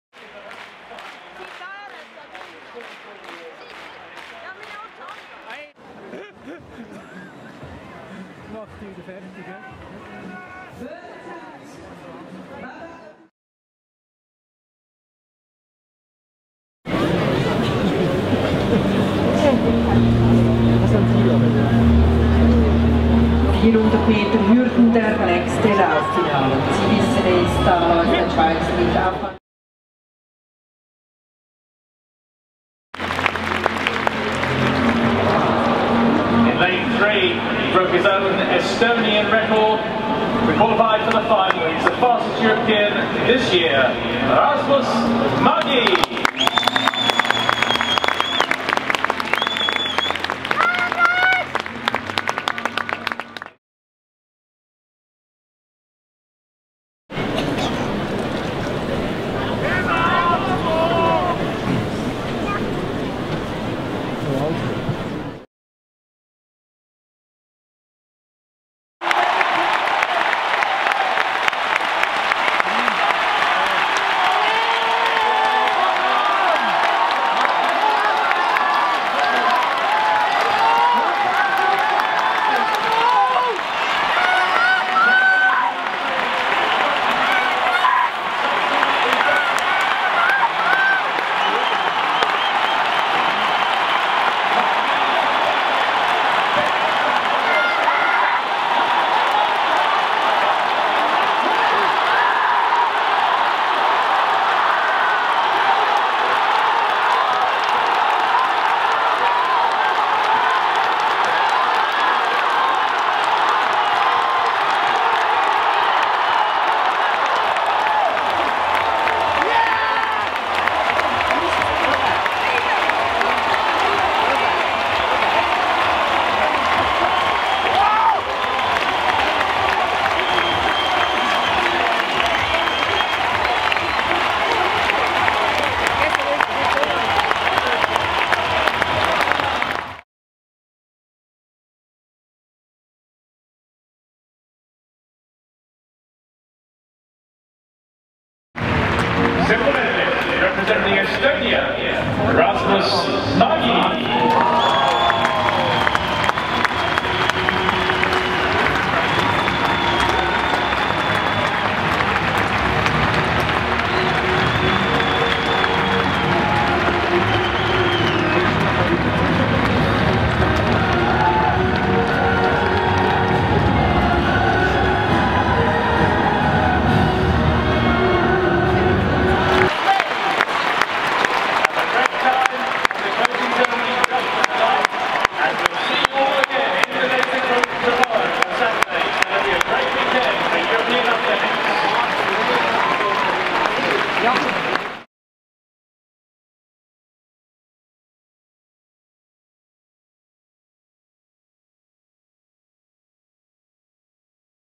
Rasmus Mägi in Zürich (2014 European Athletics Championships).
The best Estonian male athlete of the year 2014 is a 400 m hurdle runner Rasmus Mägi. He renewed Estonian national record three times and won a silver medal in Zürich 2014 European Athletics Championships.

Eesti aasta parim meeskergejõustiklane on 400 m tõkkejooksja Rasmus Mägi. Ta uuendas Eesti rekordit kolmel korral ja võitis hõbemedali Zürichi Euroopa meistrivõistlustel.